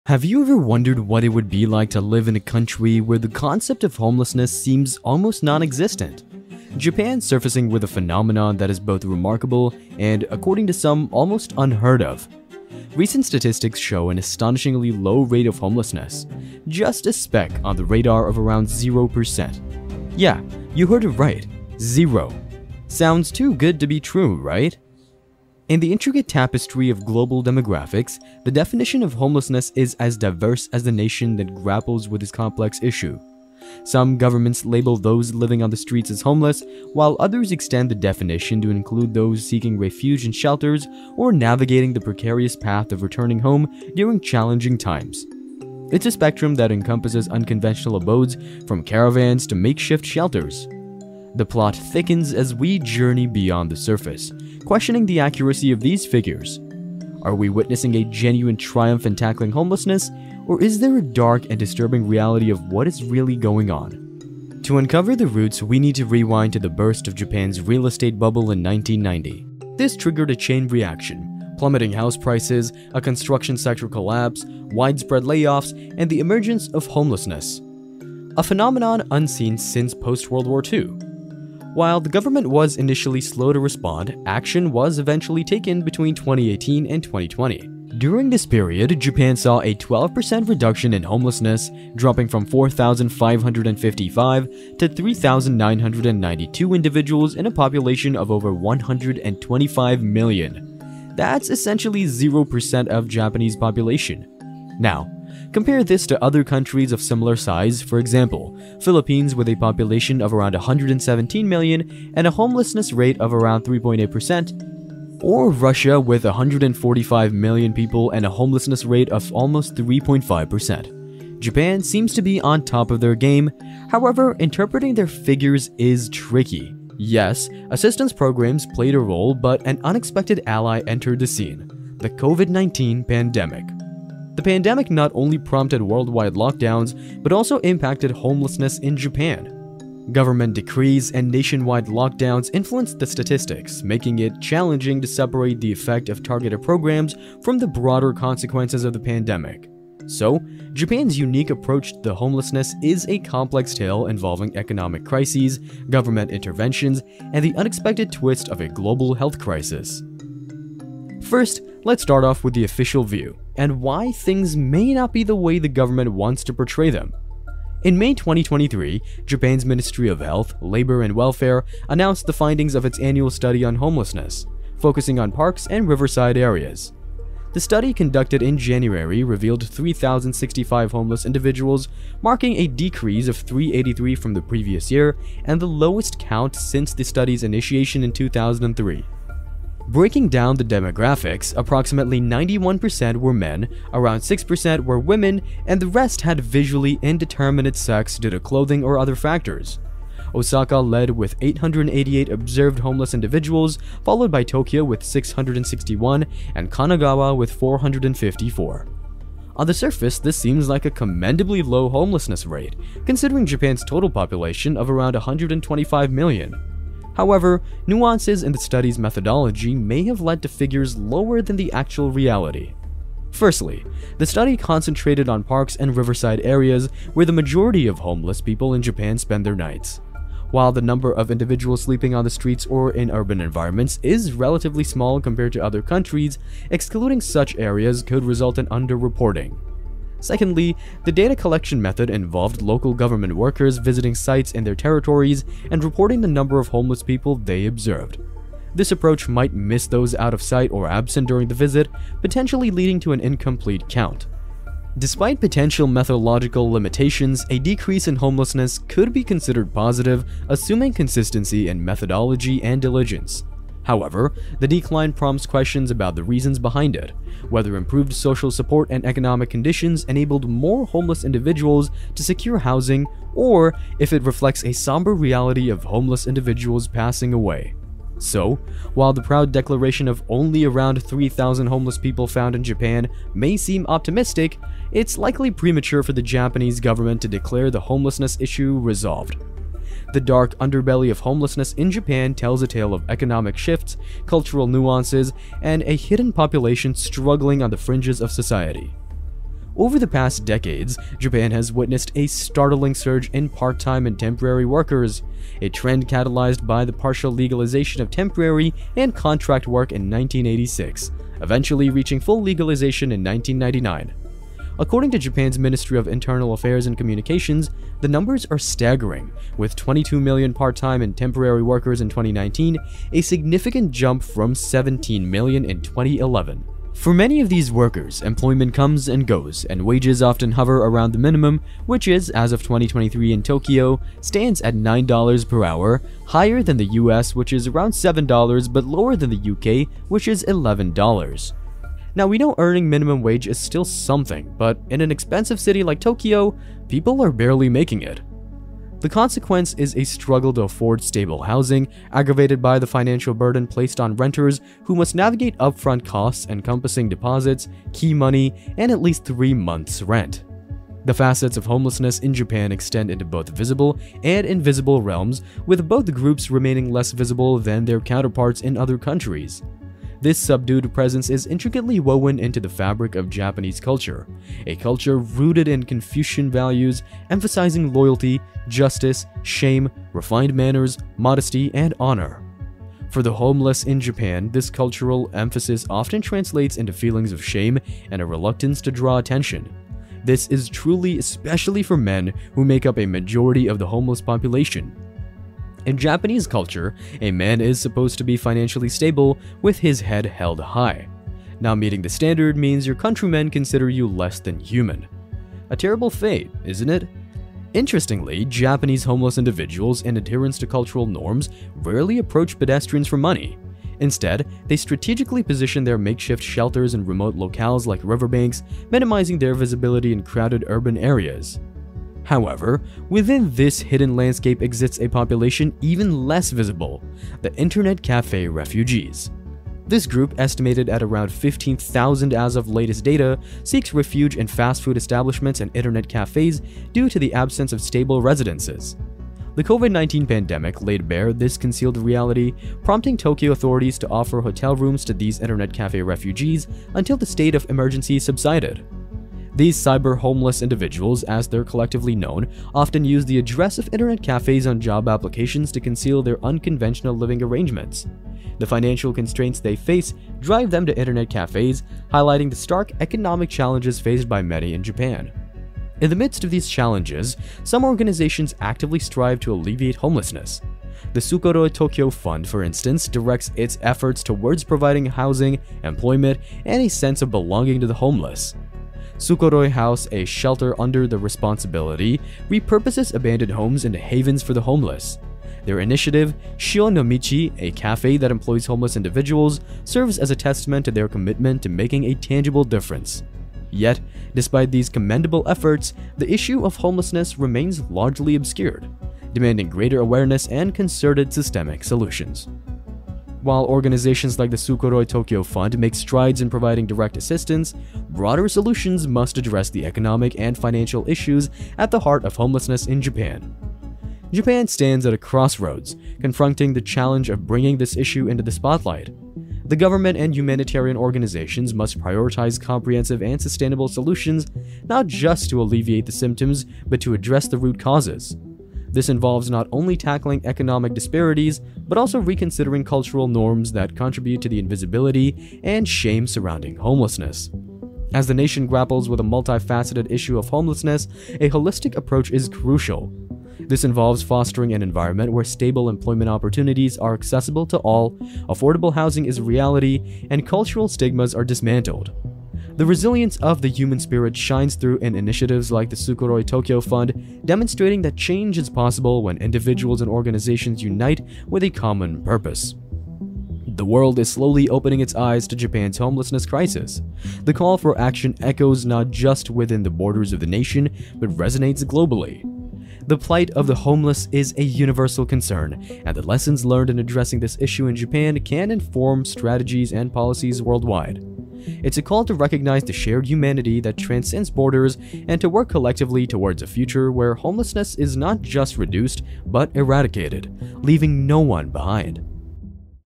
What Japan Doesn’t Tell You About Its 0% Homelessness Rate
Have you ever wondered what it would be like to live in a country where the concept of homelessness seems almost nonexistent? Japan surfacing with a phenomenon that is both remarkable and, according to some, almost unheard of. Recent statistics show an astonishingly low rate of homelessness - just a speck on the radar of around 0%.

Yes, you heard it right, zero. Sounds too good to be true, right? So how did Japan manage to have 0% homelessness rate. Dive into the story behind it with us and explore the dark side. Because if something seems too good to be true, it usually is.